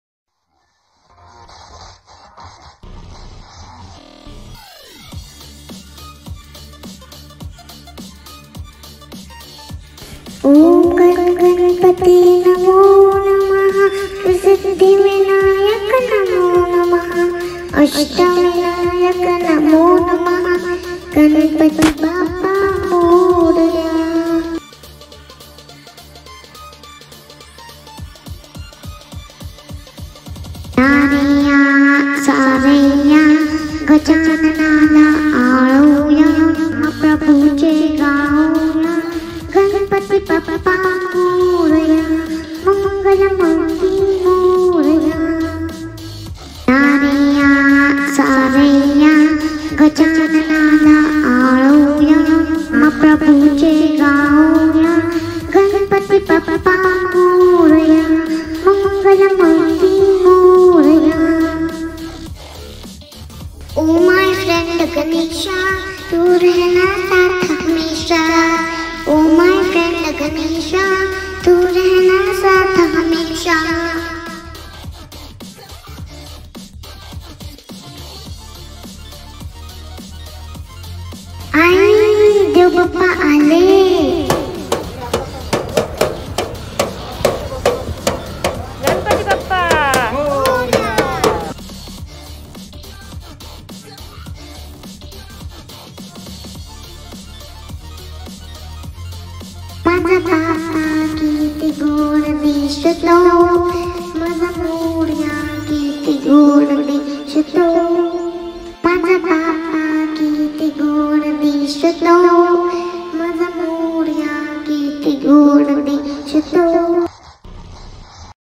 Ganpati Celebrations || Olympic Theme Decoration || Eco Friendly Ganesha || Vaishnavi Garje & Family
Art and craft done at home
DIY
Ganesh Festival
Shadu Mati Ganesha

Ganeshotsav 2021